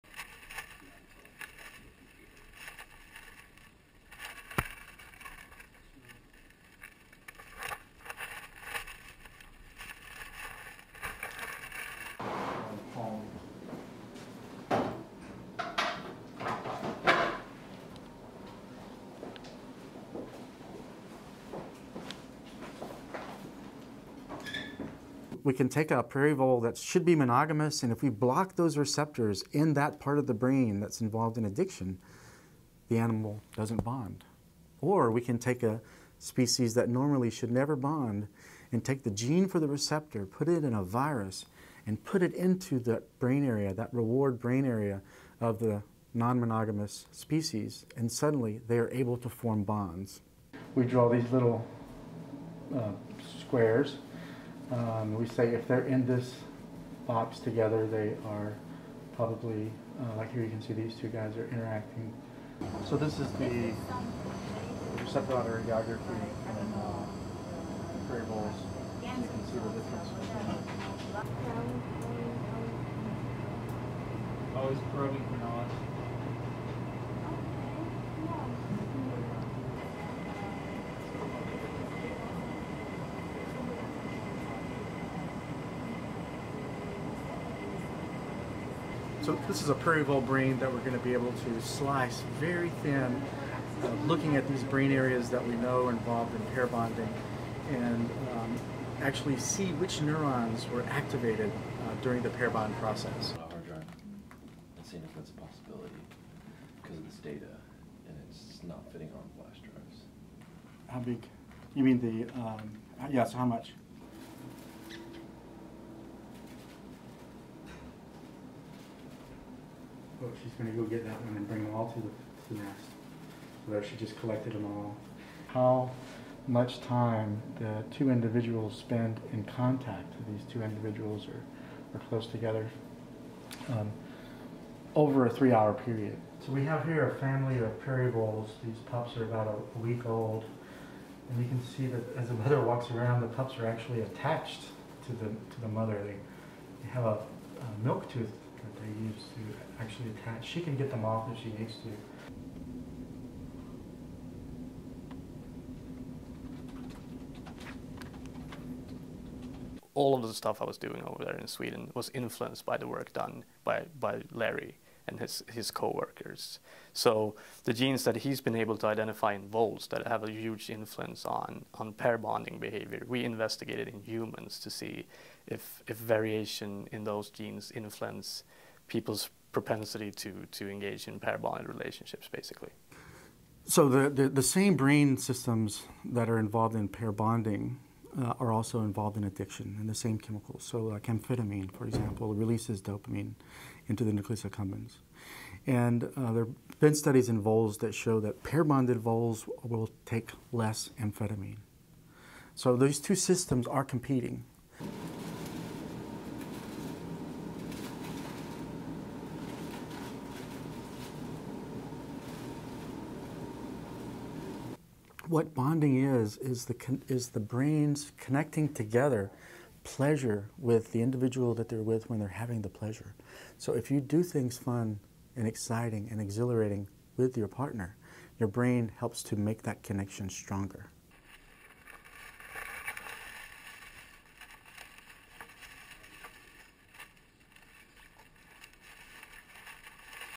Researchers explore the science behind monogamy
(19 Feb 2015) LEAD IN:
American scientists are honing in on the genetics of love, or at the very least desire by looking at the habits of voles.
The little rodents are among the very few creatures which are monogamous in the wild, but researchers say a treatment with a hormone can turn them into prosmiscuous Casanovas who like to play the field.
STORY-LINE:
These little animals are prairie voles.
Cute, furry and according to scientists here, they're normally faithful.
Once they mate, it's usually for life.
They're sociable creatures, but not promiscuous.
That's not the case with most of the animal kingdom, who unlike humans are driven to mate as often and as widely as they can.
But scientists here at the Department of Medicine at Emory University have discovered a way to make them want to play the field.
Dr Larry Young and his team are researching receptors in the brain to change the mating habits of voles.
The researchers took the hormone vasopressin and put it in a viral vector which they injected into their brains.
"We can take a prairie vole that should be monogamous and if we block those receptors in that part of the brain that's involved in addiction the animal doesn't bond. Or we can take a species that normally should never bond  and take the gene for the receptor, put it in a virus and put it in to the brain area, that reward brain area of the non-monogamous species and suddenly they're able to form bonds."
So have scientists uncovered the secrets of desire?
Young believes the research has offered up some interesting insights into the chemicals acting in the brain and how they're activating neural circuits which create desire for a relationship between individuals, even a bond which can last a lifetime.
By manipulating levels of the hormone vassopressin they can make overly amorous voles mate with just one partner.
Page explains: "So, this is a prairie vole brain that we're going to be able to slice very thin, looking at these brain areas that we know are involved in pair bonding and actually see which neurons are activated during the pair bond process."
Studying animals is one thing, but can the same study be applied to humans?
One of the scientists in Young's laboratory is a Swede who says Young's work is attracting international interest.
Post doctoral researcher Hasse Walum says: "All of the stuff I was doing over there in Sweden was influenced by the work done by Larry and his co-workers. So, the genes that he's been able to identify in voles that have a huge influence on pair bonding behaviour, we investigated in humans to see variation in those genes influence people's propensity to engage in pair bond relationships basically."
Young believes his finding in voles could be significant.
He argues the same emotions which are involved in human romantic relationships involve the same chemicals and brain structures as they are discovering in the pair bonding in voles.
Young says: "So, the same brain systems that are involved in pair bonding are also involved in addiction, and the same chemicals. So like amphetamine, for example, releases dopamine into the nucleus accumbens. And there have been studies in voles that show that pair-bonded voles will take less amphetamine. So these two systems are competing."
According to Young: "What pair bonding is is the brain's connecting together pleasure with the individual they're with when they're having the pleasure. So if you do things fun and exciting and exhilarating with your partner, your brain helps to make that connection stronger."